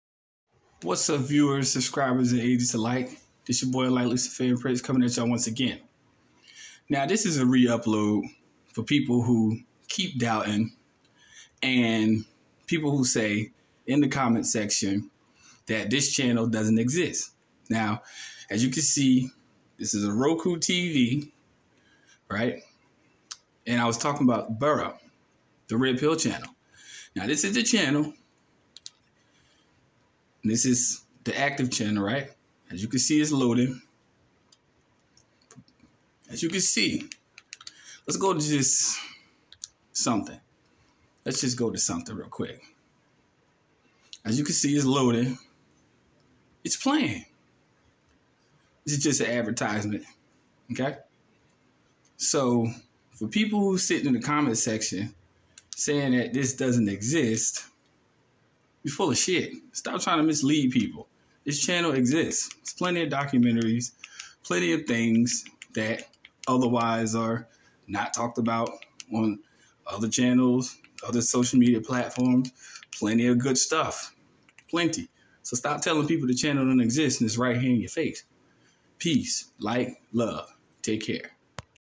Burrow: The Red Pill Channel (Reupload)
This video is to debunk those in the comments section that this channel still exists... For the Agents saying that it doesn't...

 Here's living proof in 2023!

 Until then, I Am...

 Lord Enlightened Luciferian Prince 7.7